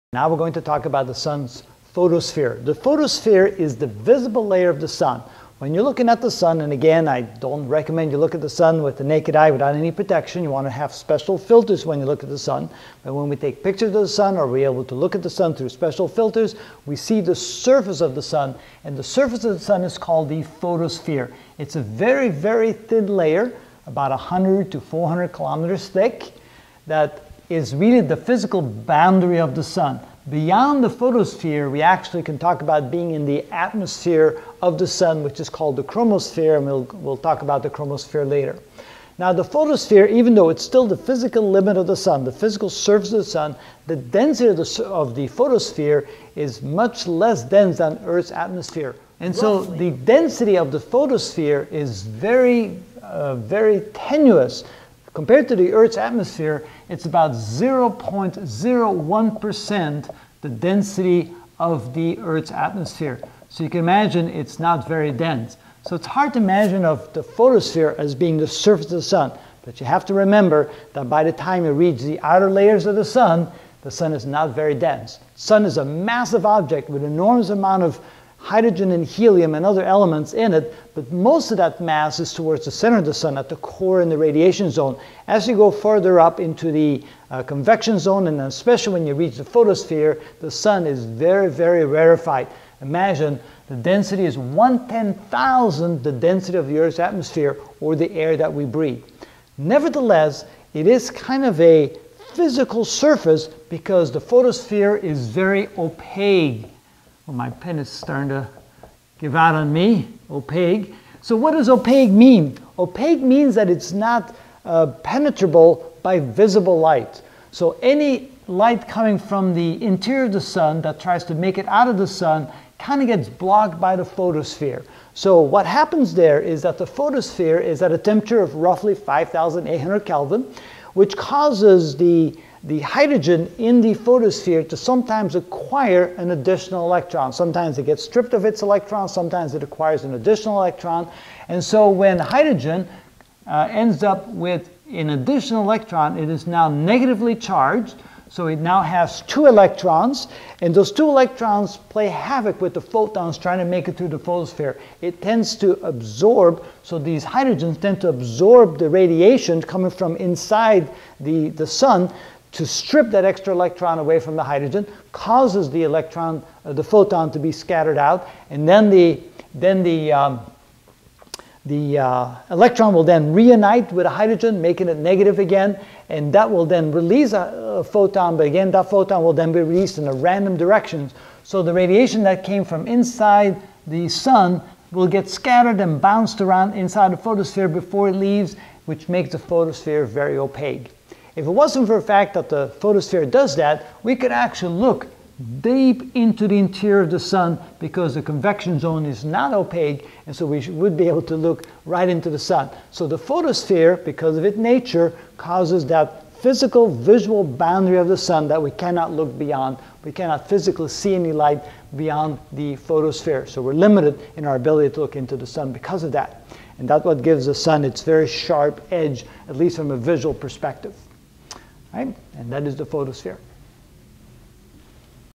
Astronomy - The Sun (11 of 16) The Photosphere
In this video I will explain the photosphere.